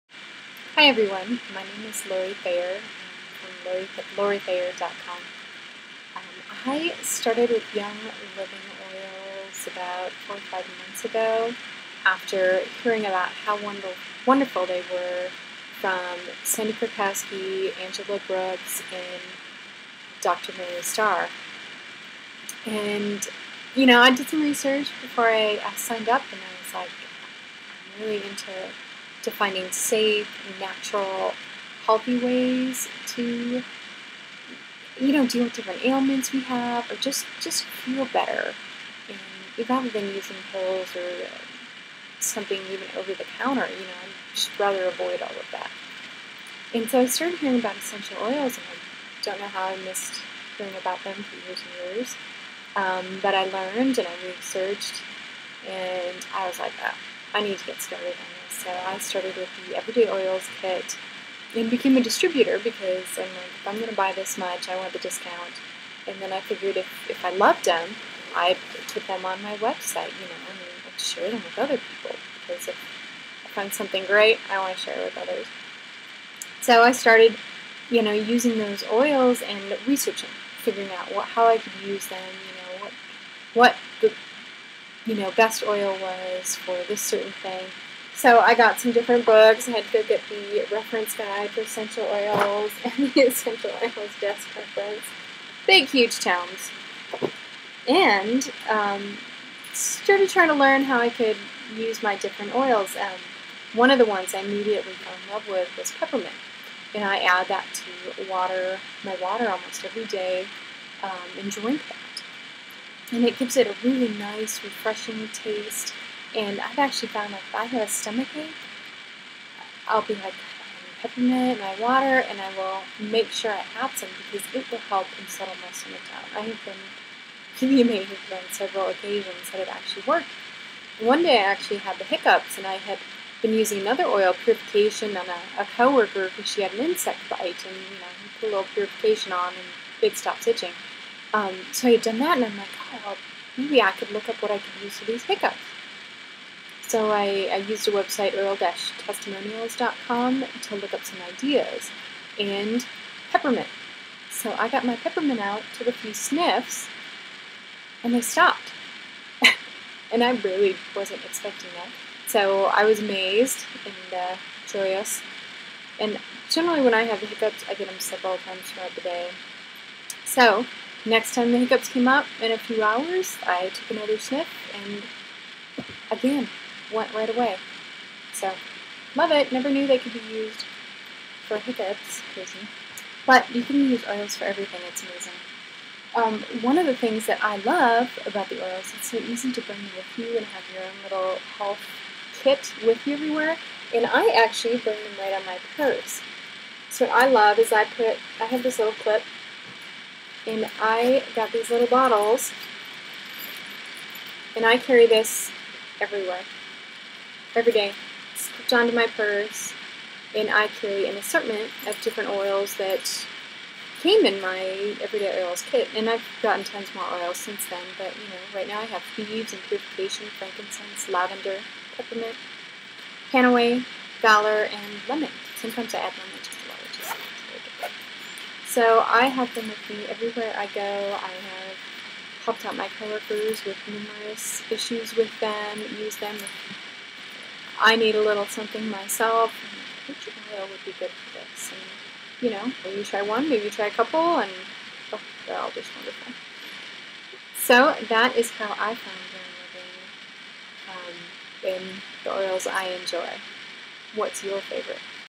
Peppermint Essential Oil & Portable Health Kit
Peppermint Essential Oil is great for stomach aches, hiccups and many other uses. It is one of eight oils I carry with me every day in my portable health kit.

In this video I'll share with you how I found Young Living Essential Oils and how I am using them in my everyday life.